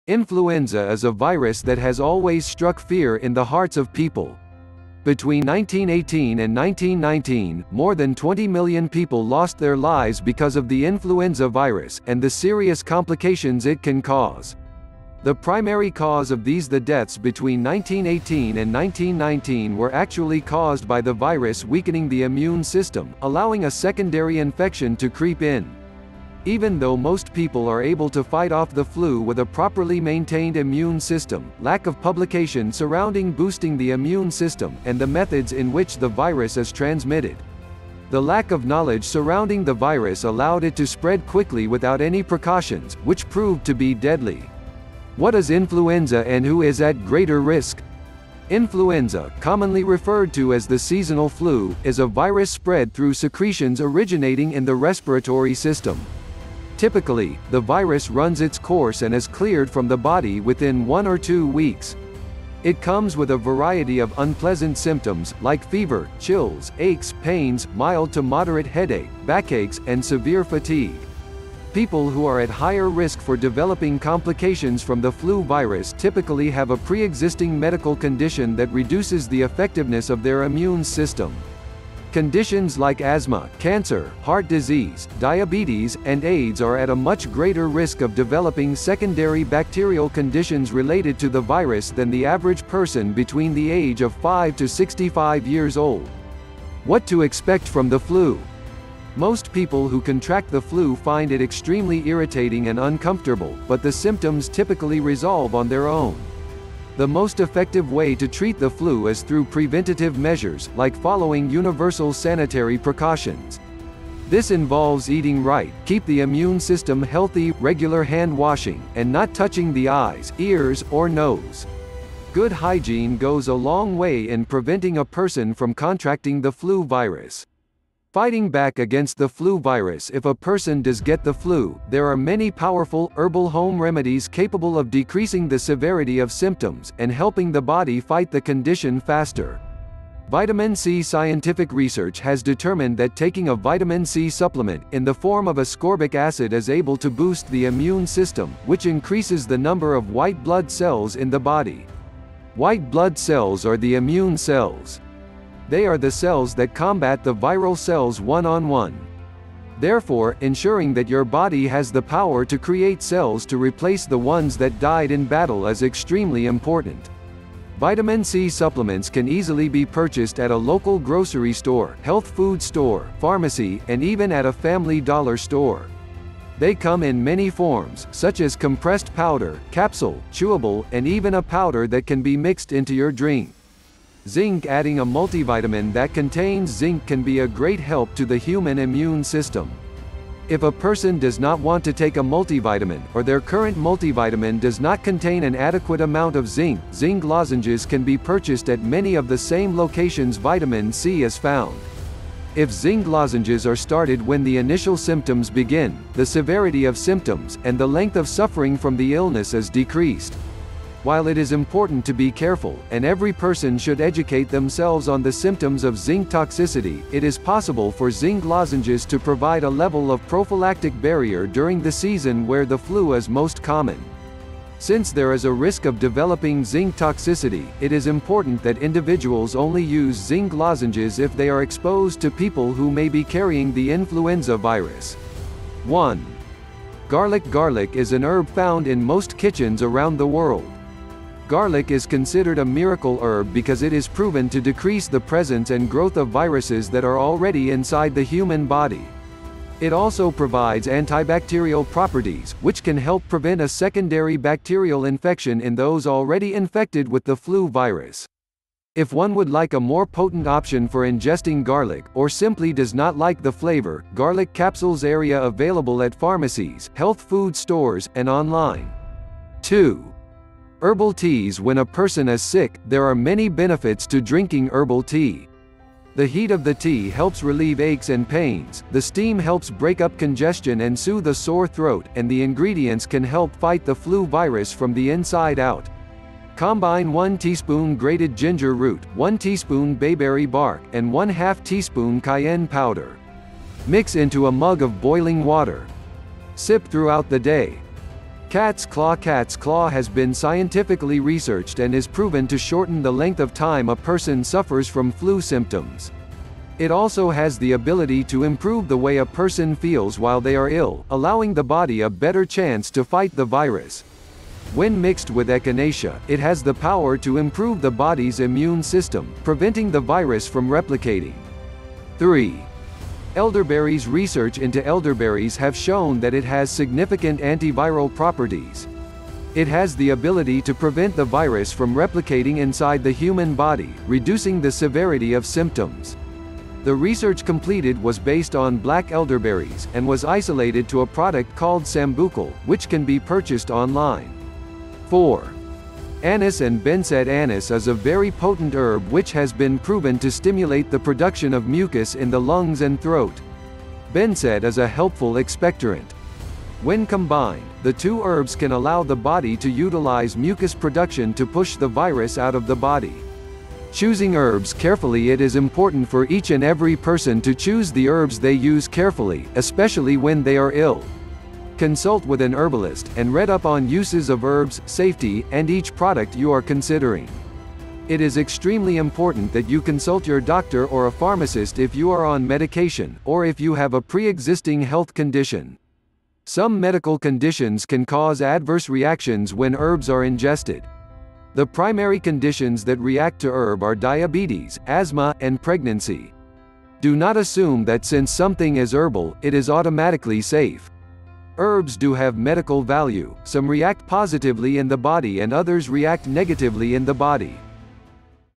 Herbal medicine for flu and colds
Most people who contract the flu find it extremely irritating and uncomfortable, but the symptoms typically resolve on their own. The most effective way to treat the flu is through preventative measures, like following universal sanitary precautions. This involves eating right, keep the immune system healthy, regular hand washing, and not touching the eyes, ears, or nose. Good hygiene goes a long way in preventing a person from contracting the flu virus.
Fighting Back Against the Flu Virus
If a person does get the flu, there are many powerful, herbal home remedies capable of decreasing the severity of symptoms, and helping the body fight the condition faster.
Vitamin C
Scientific research has determined that taking a vitamin C supplement, in the form of ascorbic acid is able to boost the immune system, which increases the number of white blood cells in the body. White blood cells are the immune cells. They are the cells that combat the viral cells one on one. Therefore, ensuring that your body has the power to create cells to replace the ones that died in battle is extremely important.
Vitamin C supplements can easily be purchased at a local grocery store, health food store, pharmacy, and even at a Family Dollar Store. They come in many forms, such as compressed powder, capsule, chewable, and even a powder that can be mixed into your drink.
Zinc
Adding a Multivitamin that contains zinc can be a great help to the human immune system. If a person does not want to take a multivitamin, or their current multivitamin does not contain an adequate amount of zinc, zinc lozenges can be purchased at many of the same locations vitamin C is found. If zinc lozenges are started when the initial symptoms begin, the severity of symptoms, and the length of suffering from the illness is decreased.
While it is important to be careful, and every person should educate themselves on the symptoms of zinc toxicity, it is possible for zinc lozenges to provide a level of prophylactic barrier during the season where the flu is most common. Since there is a risk of developing zinc toxicity, it is important that individuals only use zinc lozenges if they are exposed to people who may be carrying the influenza virus.
Garlic
Garlic is an herb found in most kitchens around the world. Garlic is considered a miracle herb because it is proven to decrease the presence and growth of viruses that are already inside the human body. It also provides antibacterial properties, which can help prevent a secondary bacterial infection in those already infected with the flu virus. If one would like a more potent option for ingesting garlic, or simply does not like the flavor, garlic capsules area available at pharmacies, health food stores, and online.
Herbal Teas
When a person is sick, there are many benefits to drinking herbal tea. The heat of the tea helps relieve aches and pains, the steam helps break up congestion and soothe a sore throat, and the ingredients can help fight the flu virus from the inside out.
Combine 1 teaspoon grated Ginger root, 1 teaspoon Bayberry Bark, and ½ teaspoon cayenne powder. Mix into a mug of boiling water. Sip throughout the day.
Cats Claw
Cats claw has been scientifically researched and is proven to shorten the length of time a person suffers from flu symptoms. It also has the ability to improve the way a person feels while they are ill, allowing the body a better chance to fight the virus. When mixed with Echinacea, it has the power to improve the body’s immune system, preventing the virus from replicating.
Elderberries
Research into elderberries have shown that it has significant antiviral properties. It has the ability to prevent the virus from replicating inside the human body, reducing the severity of symptoms. The research completed was based on black elderberries, and was isolated to a product called Sambucol, which can be purchased online.
Anise and Beneset
Anise is a very potent herb which has been proven to stimulate the production of mucus in the lungs and throat. Beneset is a helpful expectorant. When combined, the two herbs can allow the body to utilize mucus production to push the virus out of the body.
Choosing Herbs Carefully
It is important for each and every person to choose the herbs they use carefully, especially when they are ill. Consult with an herbalist, and read up on uses of herbs, safety, and each product you are considering.
It is extremely important that you consult your doctor or a pharmacist if you are on medication, or if you have a preexisting health condition. Some medical conditions can cause adverse reactions when herbs are ingested. The primary conditions that react to herb are diabetes, asthma, and pregnancy.
Do not assume that since something is herbal, it is automatically safe. Herbs do have medical value, some react positively in the body and others react negatively in the body.